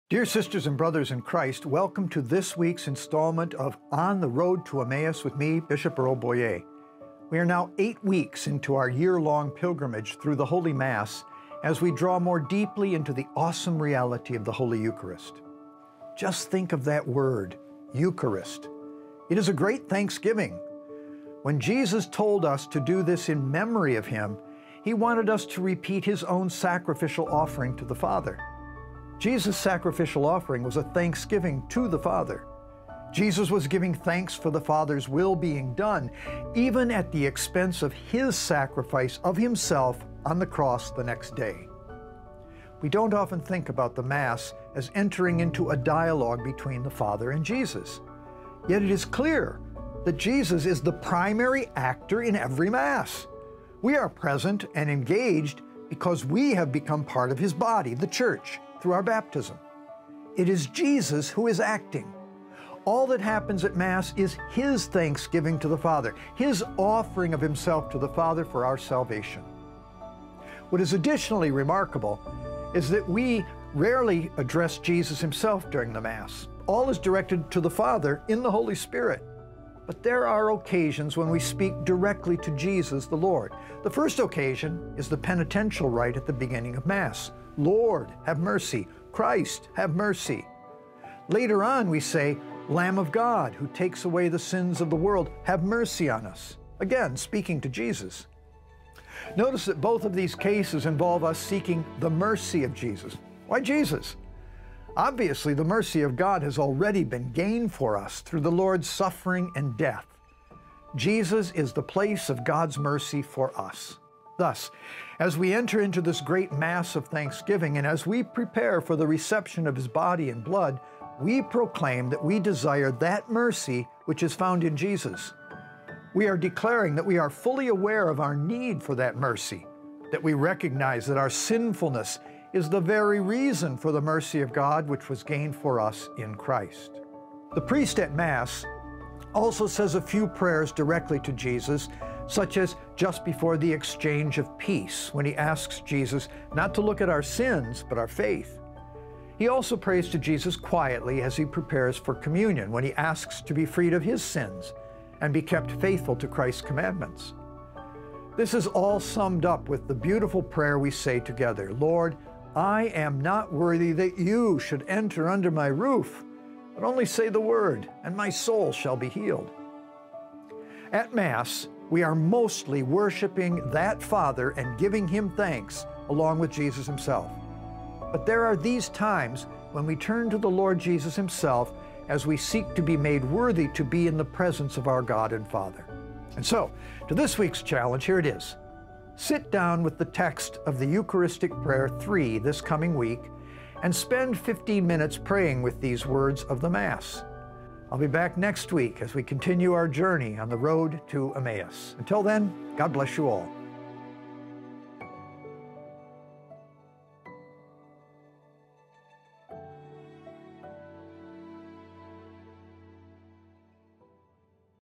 On the Road to Emmaus w/ Bishop Boyea | Week 8 | August 6 to 12
Welcome to Week 8 of On the Road to Emmaus with Bishop Earl Boyea of Lansing who is leading us, week-by-week, through the Most Holy Sacrifice of the Mass. This week's theme is thanksgiving. Says Bishop Boyea:

"Just think of that word: Eucharist!  It is a great “thanksgiving.” When Jesus told us to do this in memory of him, he wanted us to repeat his own sacrificial offering to the Father. Jesus’ sacrificial offering was a thanksgiving to the Father. Jesus was giving thanks for the Father’s will being done, even at the expense of his sacrifice of himself on the cross the next day."

And so to Bishop Boyea's challenge for this week. Here it is: "Sit down with the text of Eucharistic Prayer III this coming week and spend 15 minutes praying with these words of the Mass."

* Here is a link to the text of Eucharistic Prayer III